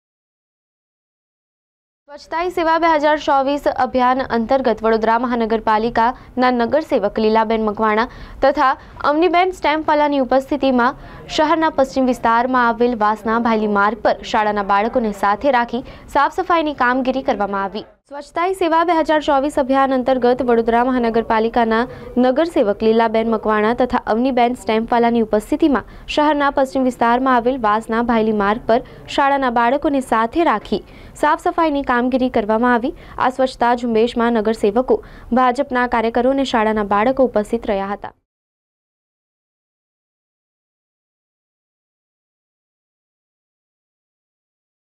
સ્વછતા હી સેવા 2024 અભિયાન અંતર્ગત વાસણા ભાયલી માર્ગ પર સાફ-સફાઈની કામગીરી 2024 | Spark Today News
સ્વછતા હી સેવા 2024 અભિયાન અંતર્ગત વાસણા ભાયલી માર્ગ પર સાફ-સફાઈની કામગીરી